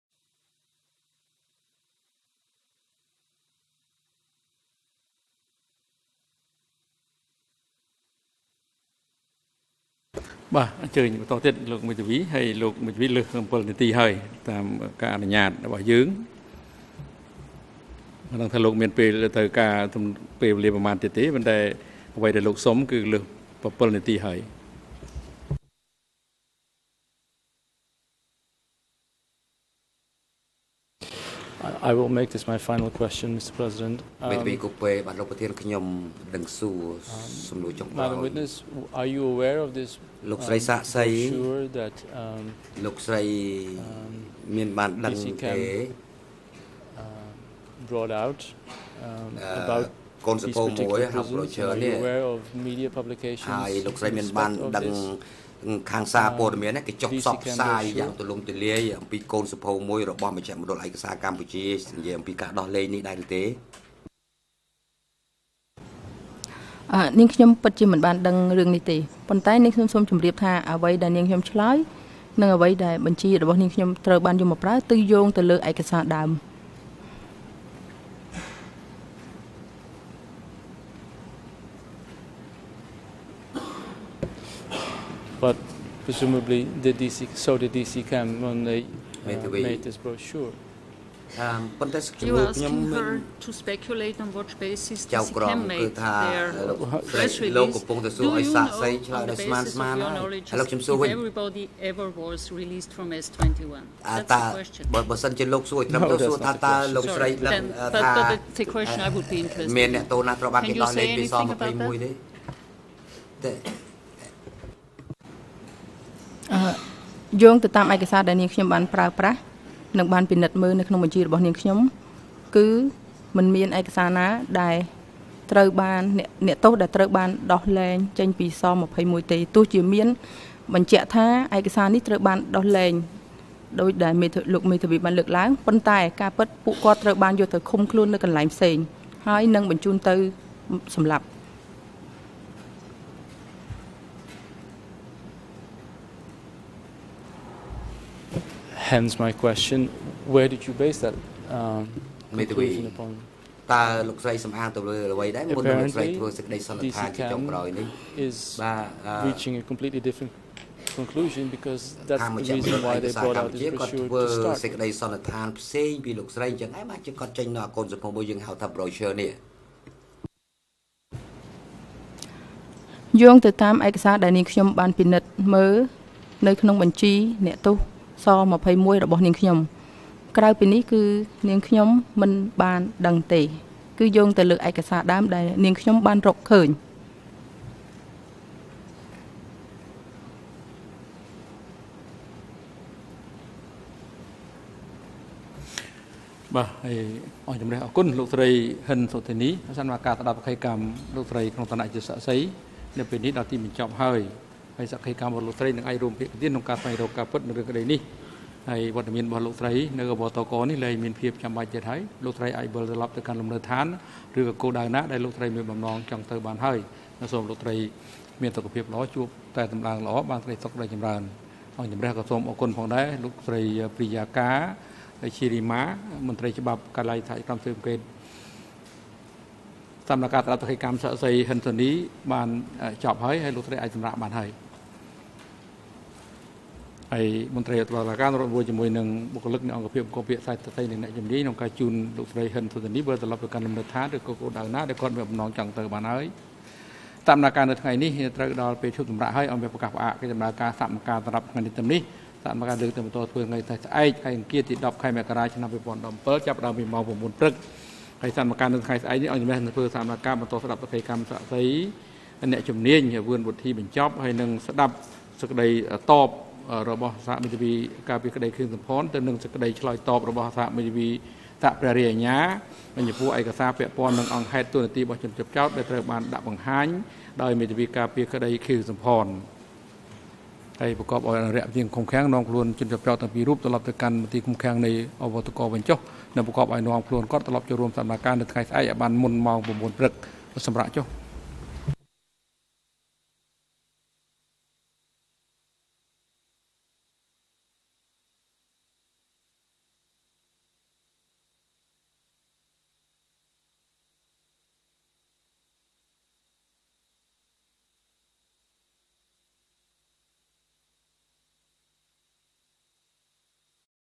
Session 3 pt.II - 5 January 2017 - Case 002/02 - FL/KH
Hearing of key documents in case 002/02.
Left Channel audio: Floor
Right channel audio: Khmer